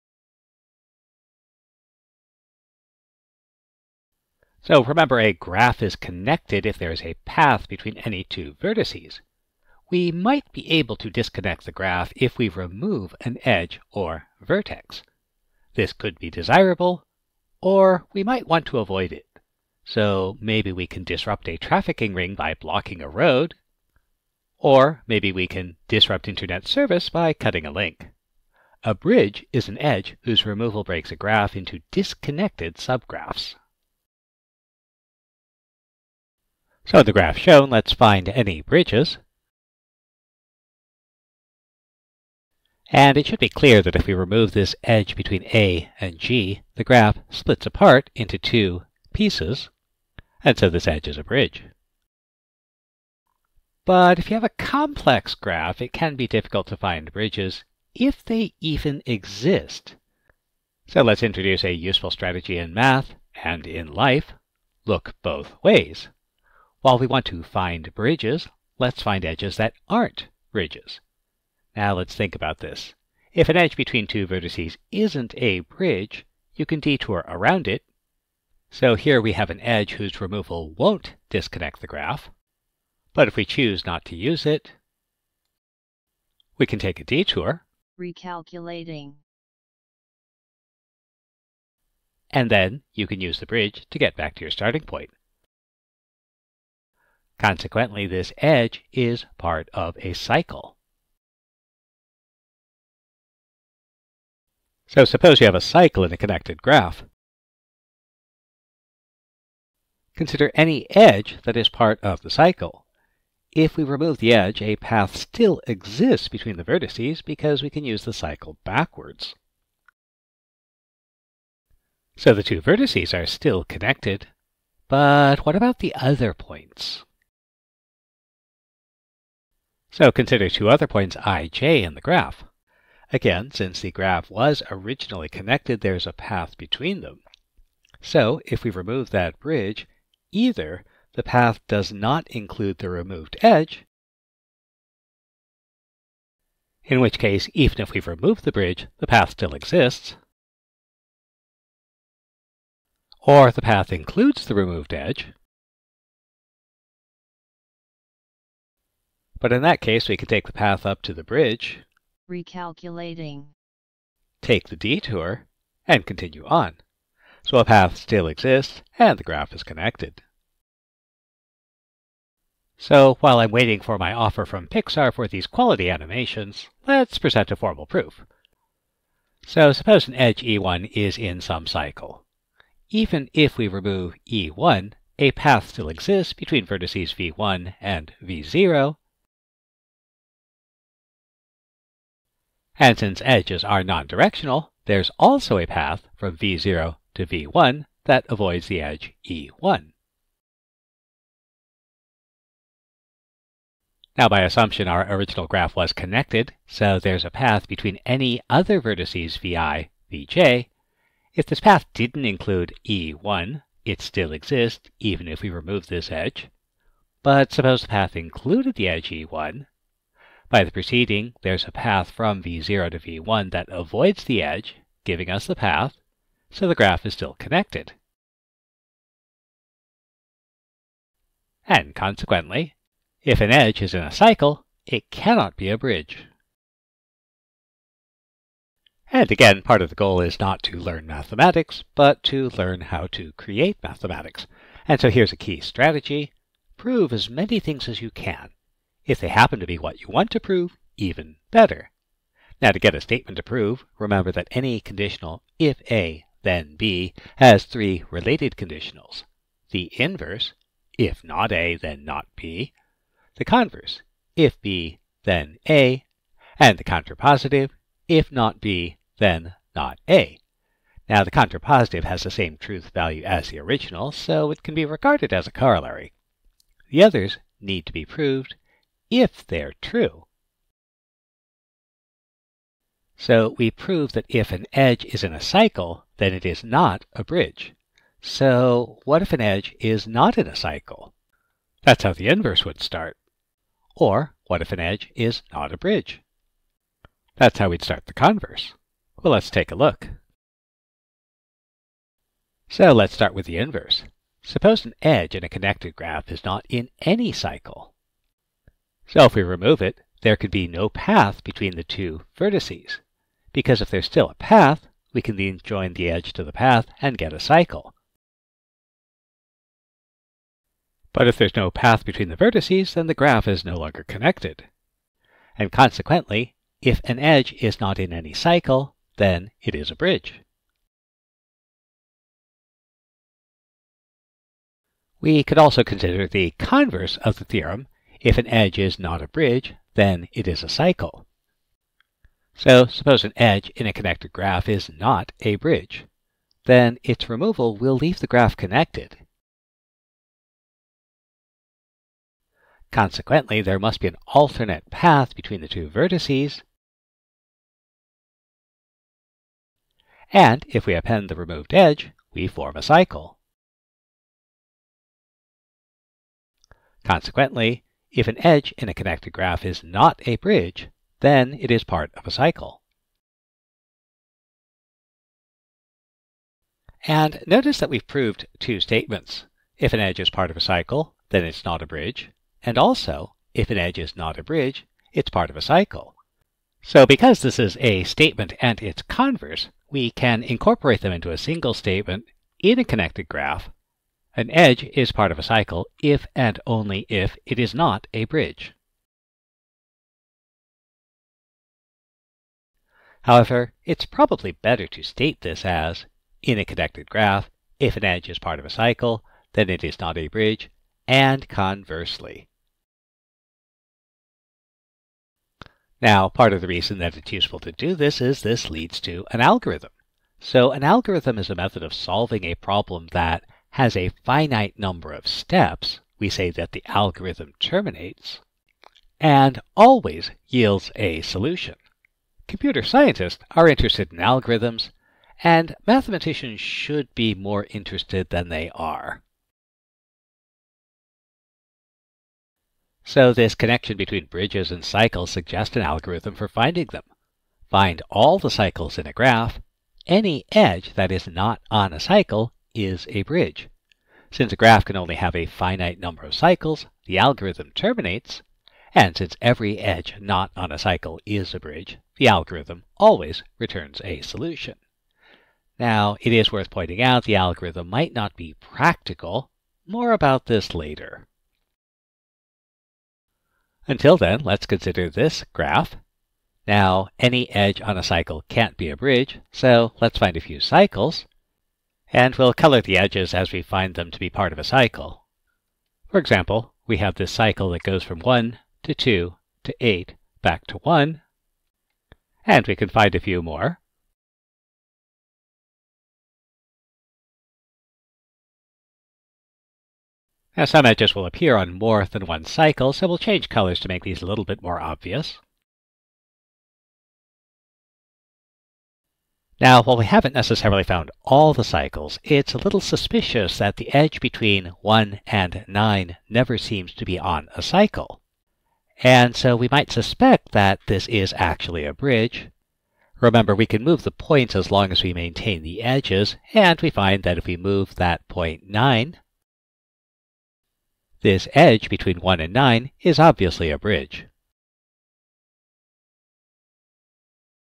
Bridges and Cycles
The relationship between bridges and cycles in a graph, and an algorithm for finding bridges.